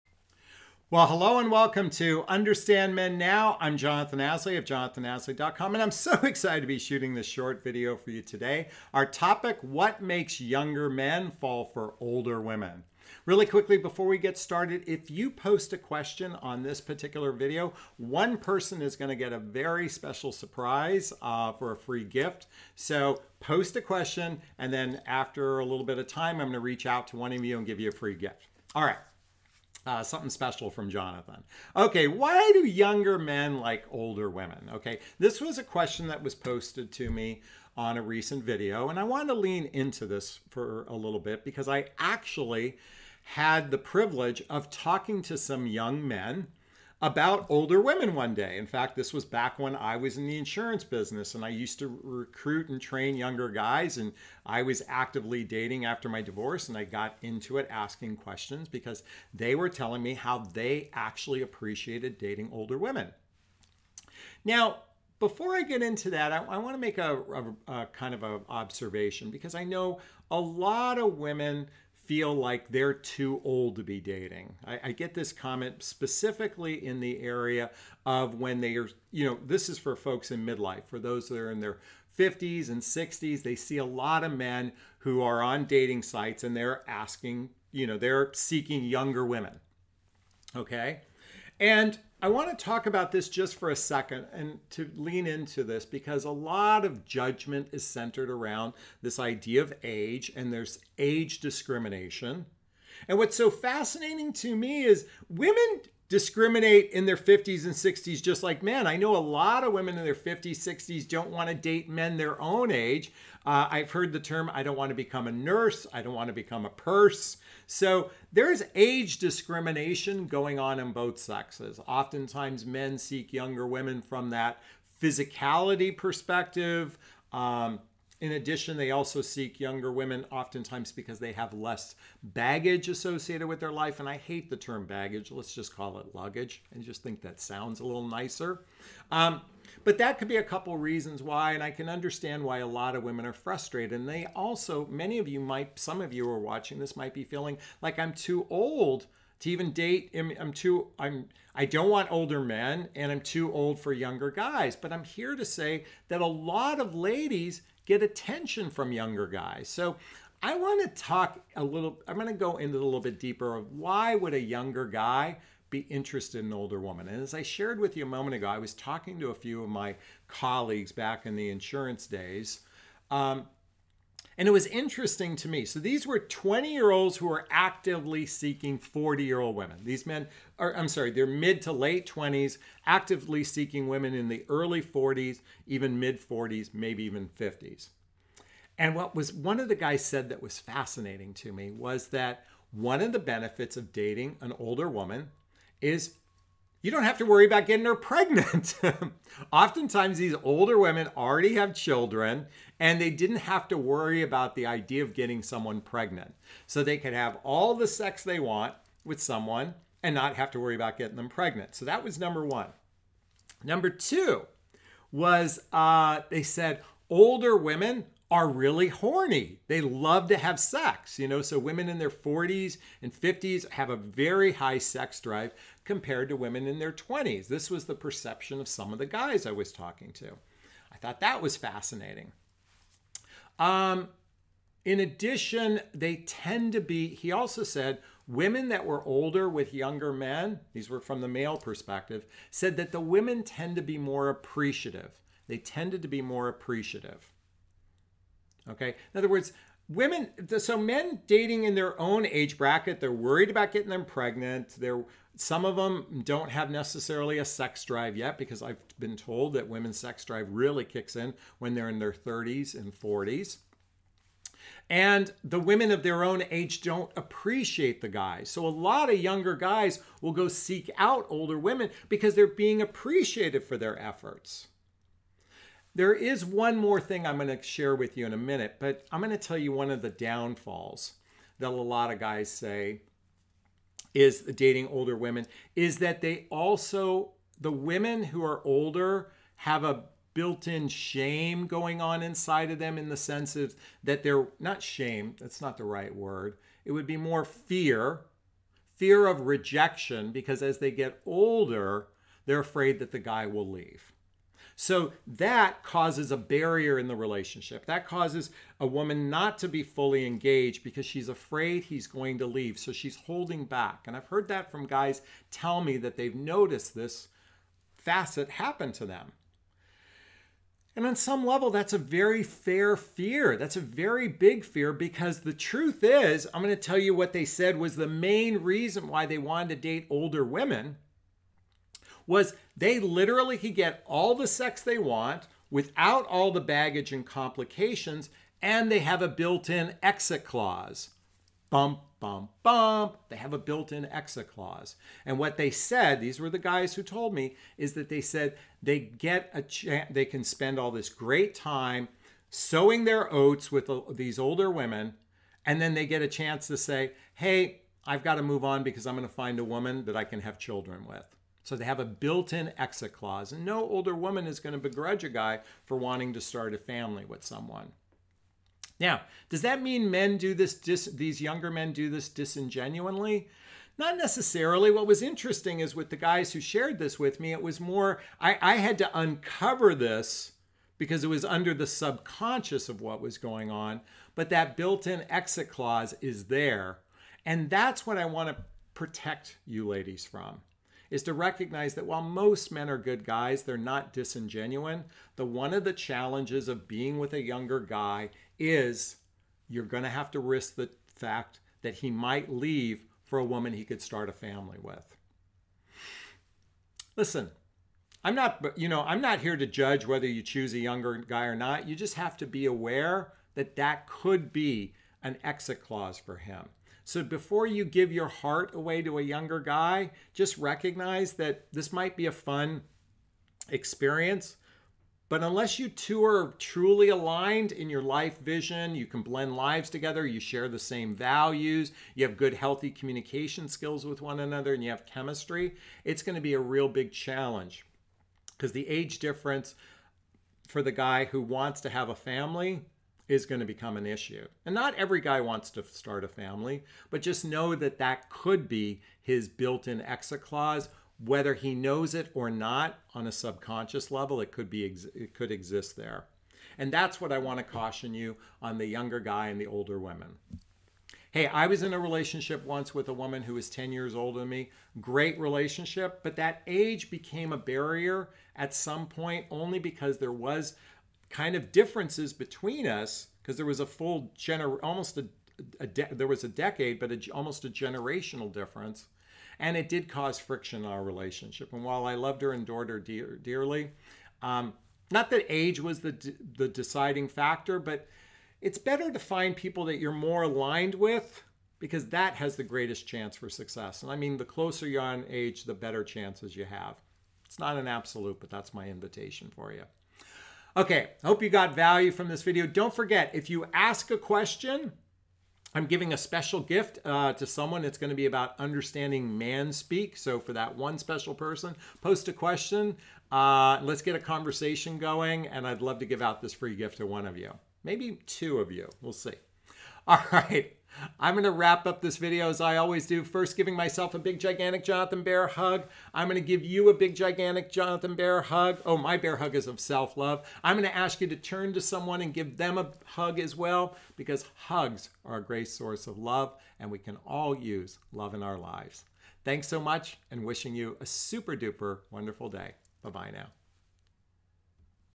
What Makes Younger Men DESIRE Older Women ~ INSIDE SCOOP
The "What Would Love Do?" Podcast

Over the years I've heard that many women feel they are too old to date because men discriminate seeking younger women.

While that is true that many men might be seeking younger women, how many of them actually even get what they want?

But here's what's often missed is that younger men are often hitting on older women and so to think you might be too old to date, I'm here to say hogwash.

Now here's the thing about younger men seeking older women, which is what I want to lean into.

In today's video, we are going to explore the DEEPER conversation about younger men and older women to be best prepared if a younger man comes your way.

Let's talk about... What Makes Younger Men DESIRE Older Women ~ INSIDE SCOOP